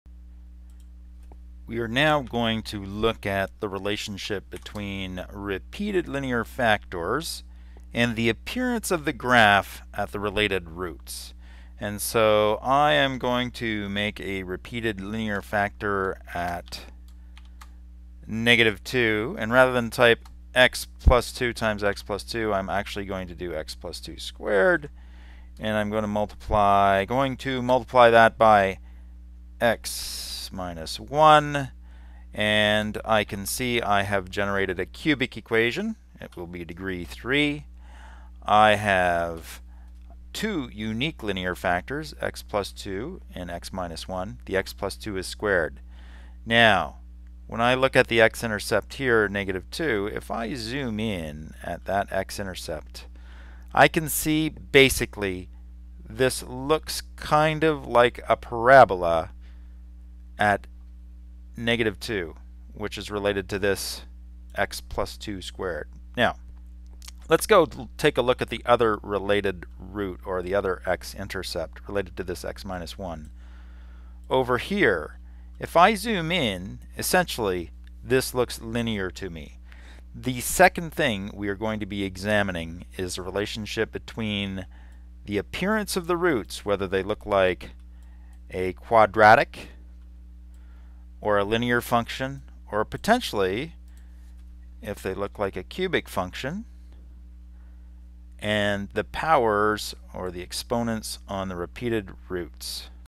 Graphs and repeated roots
What is the appearance of a graph at a repeated root?